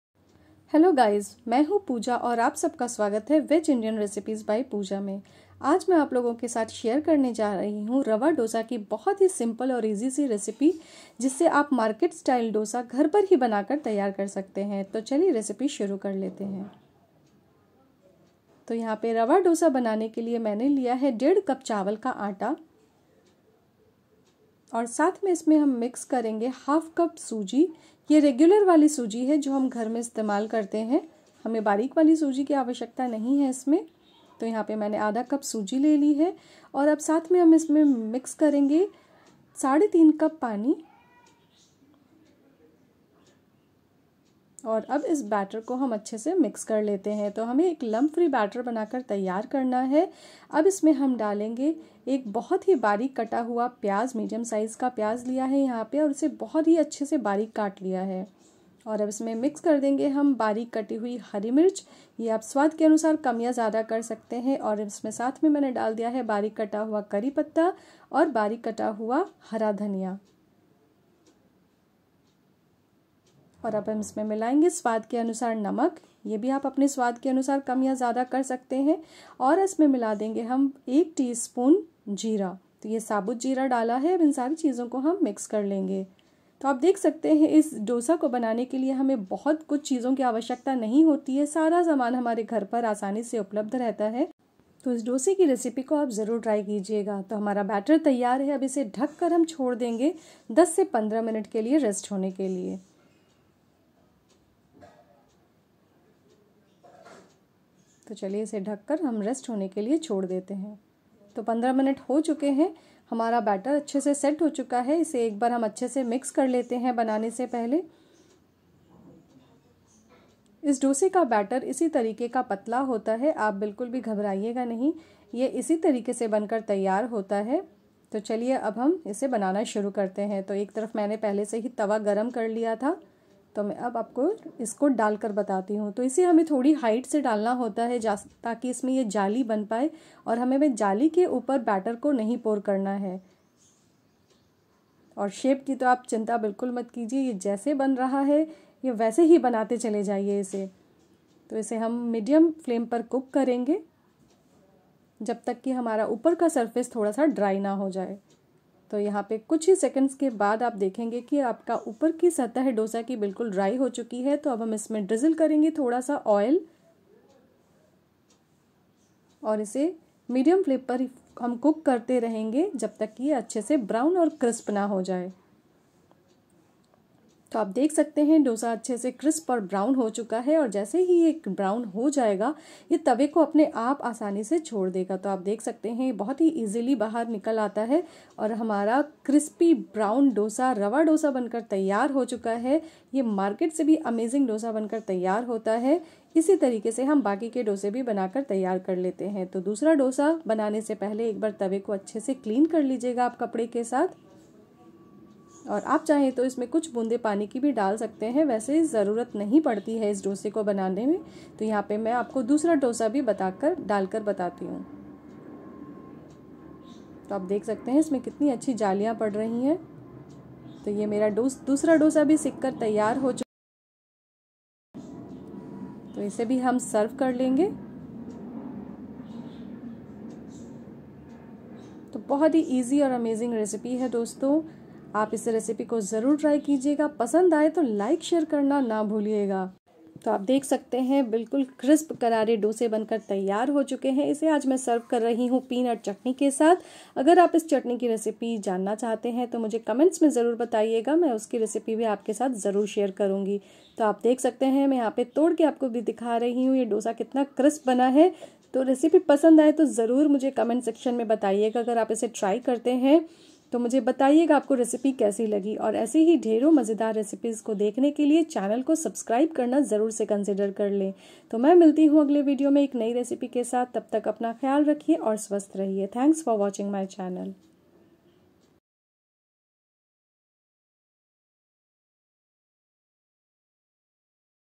Instant Rava dosa | Dosa Recipe | How To Make Rava Dosa | Suji Dosa Recipe | Best Dosa Recipe
Ingredients
Rice flour - 1 1/2 cup
Chopped coriander, curry leaves
Green chillies